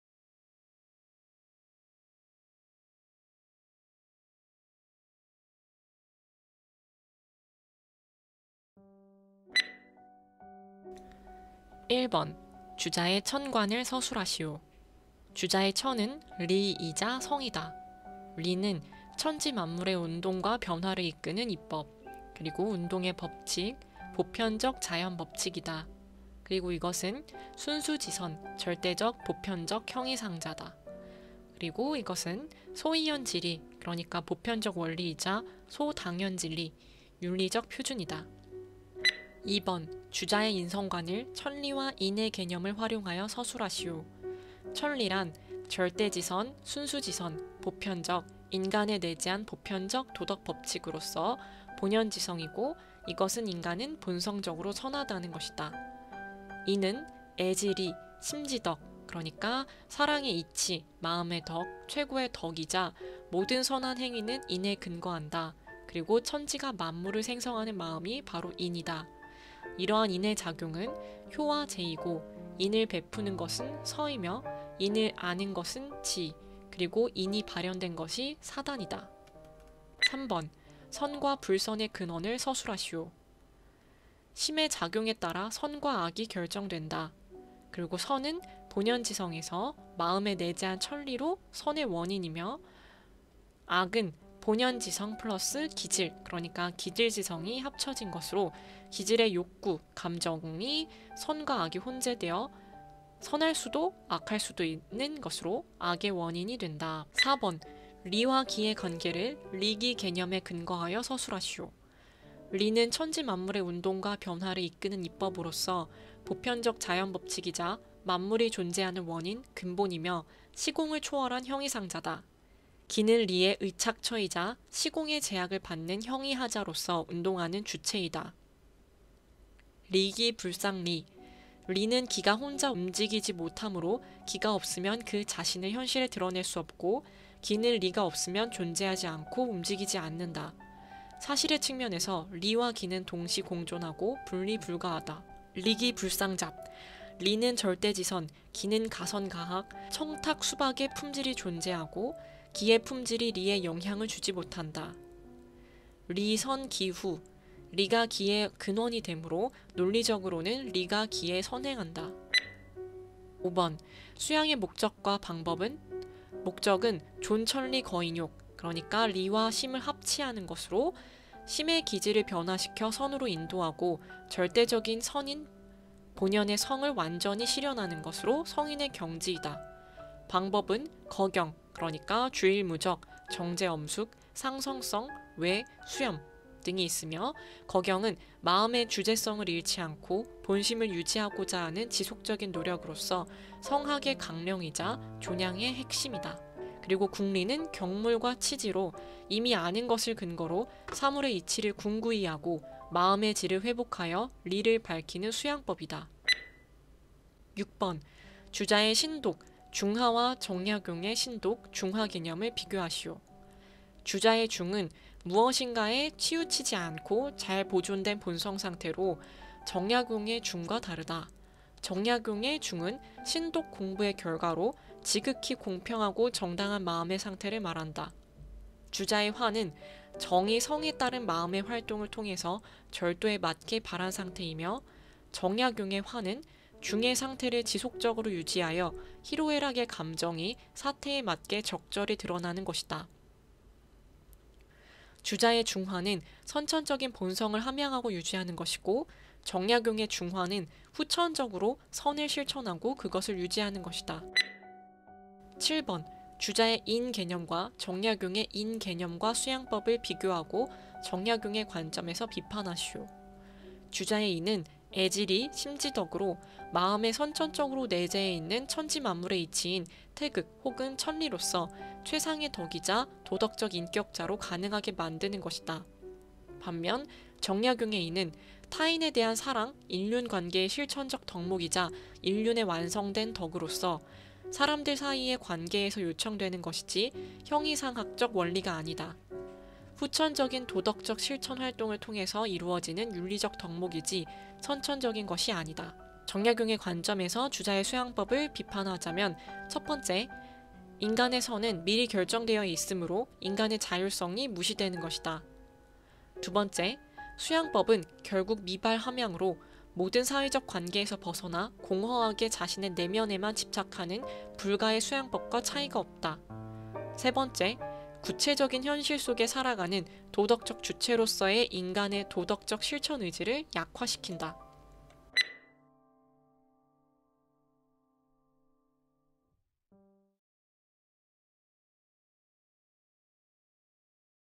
[음성주의] 주자 문제+해설영상1 / 짧은문제_긴답안 주의 / 7문제 / 잉여시간에 보기
이 시리즈들을...보고 계신 분이 계시나요..?
제가 만들었던 논제 질문들을
동영상으로 만들어서 재생목록을
가득 채우고 싶다는 욕심에
계속 하고 있기는 한데..

보는 분이 계실지 모르겠네요 ㅋㅋㅋㅋㅋㅋㅋㅋㅋㅋㅋㅋㅋㅋㅋ

이번에는 주자입니다.
2편도 있어요.  :D